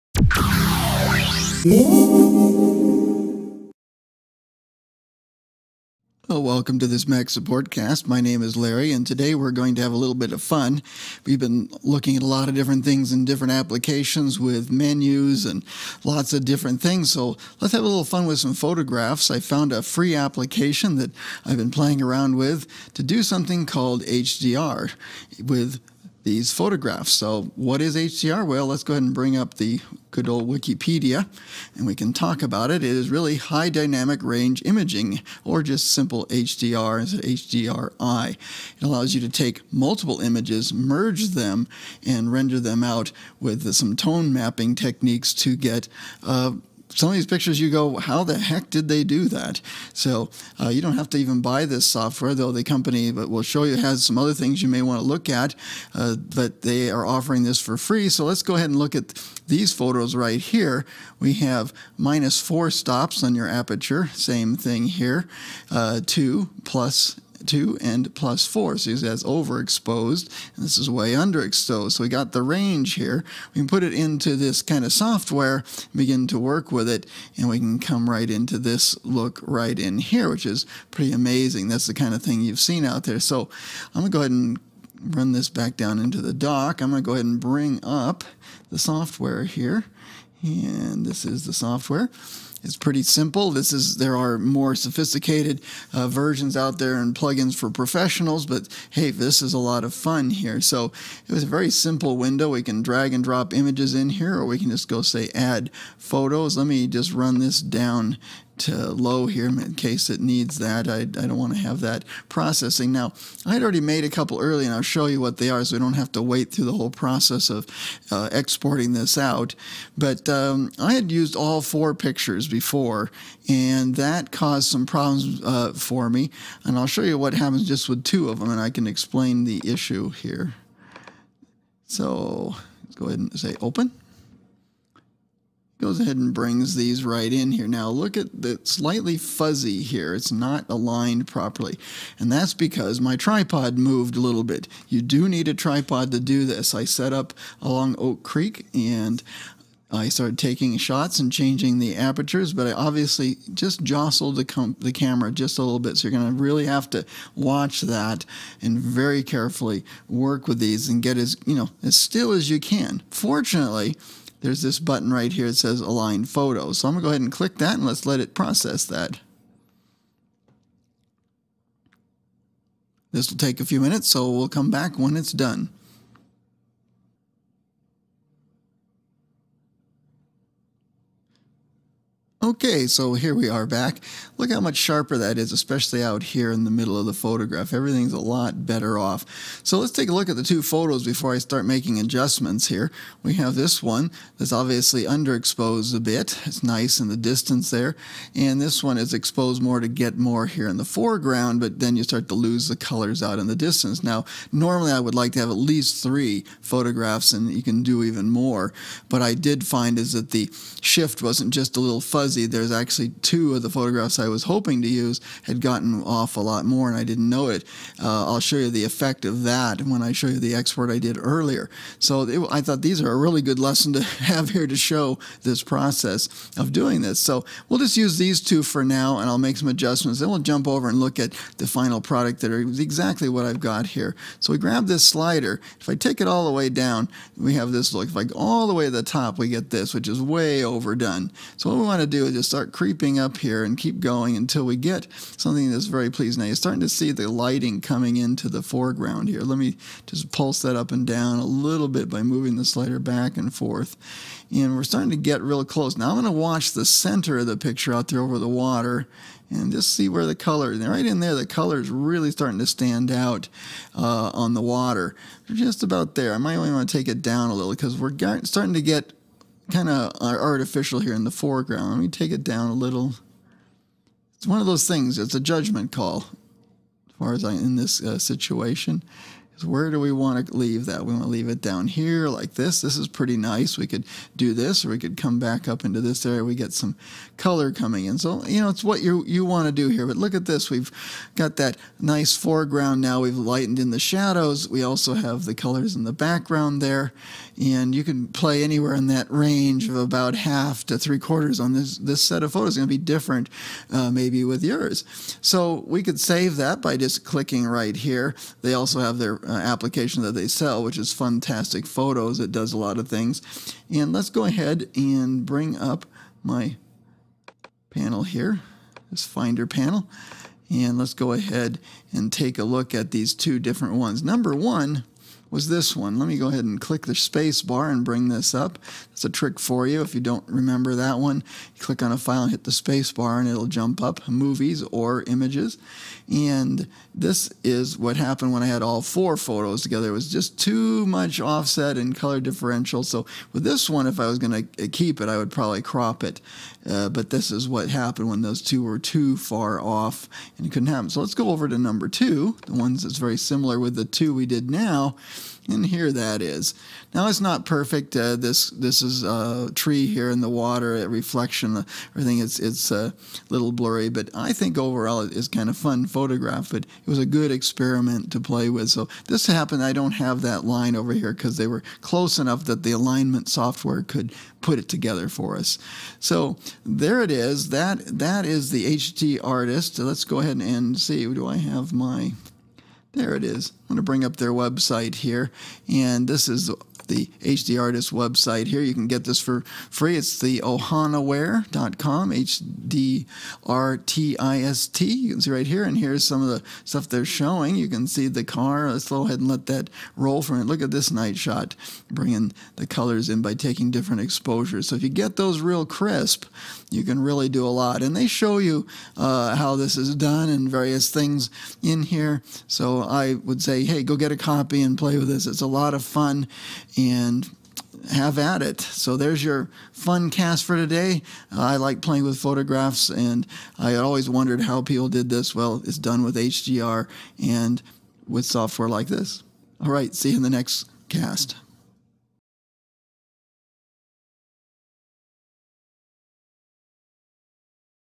Creating an HDR Photograph
you can get some quick and easy High Dynamic Range (HDR) composite images. In this tutorial I show you how to take the shots, show some issues and how easy it is to use this application.